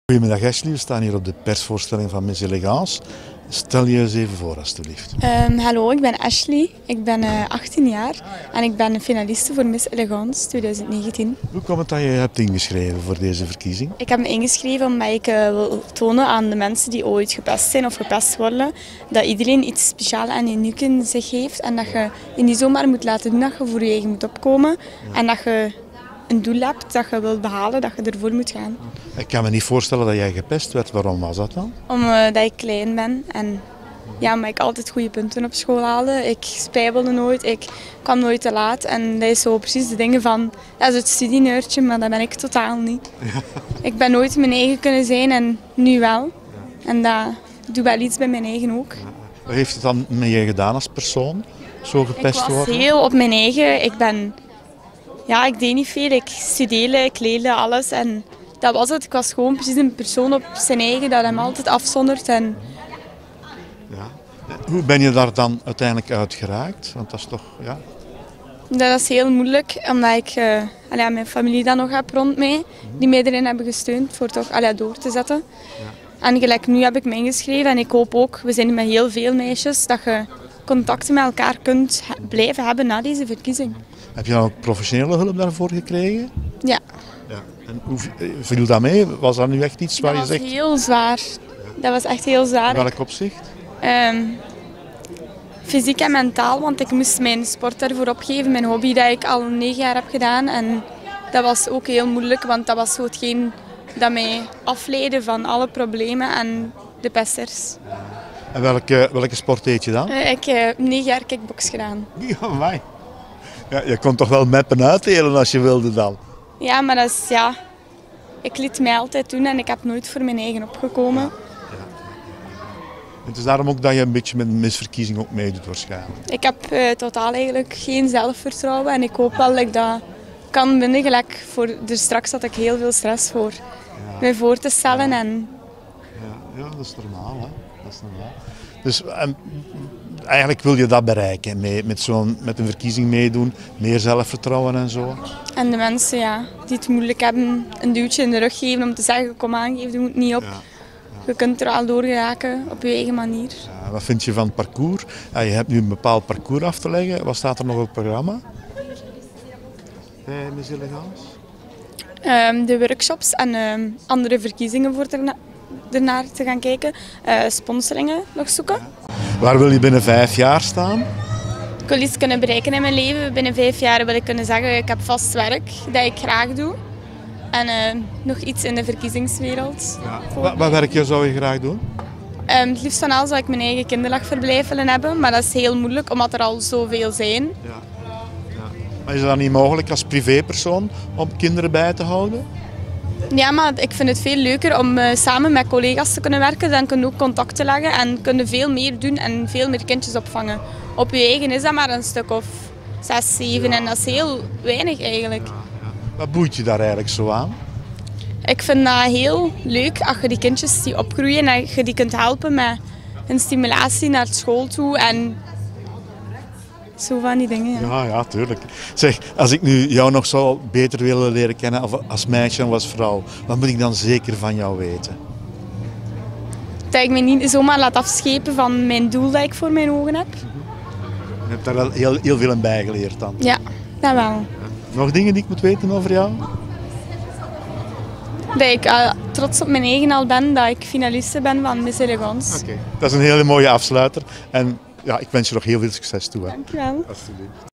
Ashley Finaliste Miss Elegance 2020
Deze 18 jarige dame verkondigt dat iedereen iets speciaal en uniek in zich heeft en dat je voor jouw doel moet gaan. Ze heeft op een gegeven moment haar kick box sport moeten opgeven die ze al negen jaar deed en ze vindt het leuk kinderen te zien opgroeien. Graag zou ze dan ook een kinderdagverblijf willen openen. In deze clip leer je haar beter kennen!